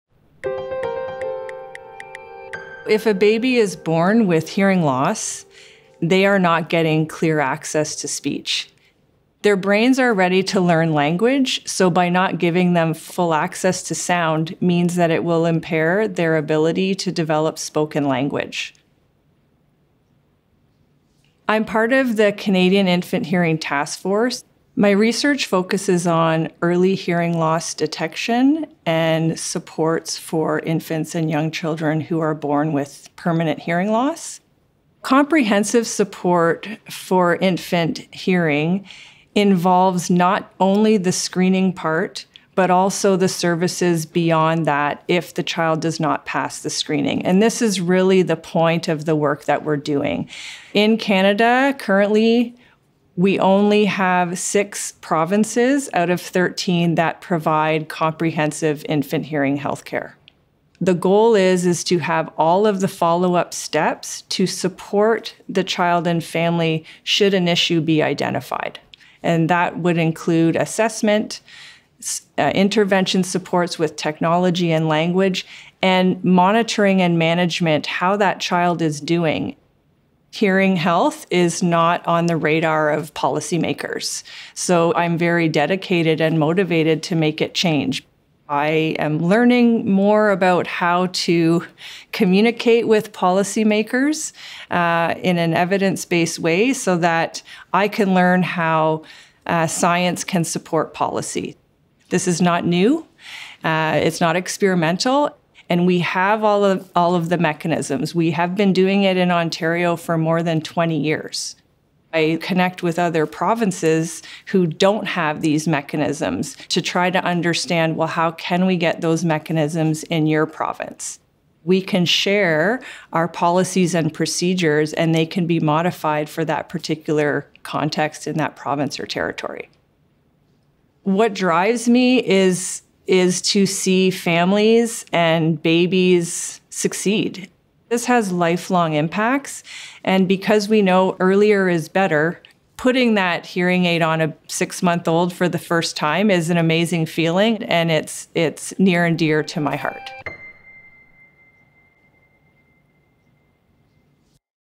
How to support children who have early hearing loss | Marlene Bagatto | The Impact Project
Join us as we uncover what it takes to make positive change in our world – featuring Marlene Bagatto, Investigator, National Centre for Audiology, Assistant Professor, Communication Sciences and Disorders, Western University. The Impact Project is a public exploration of how impact is being made in communities, cities and countries around the world.

Uploaded by: Western Communications (DL)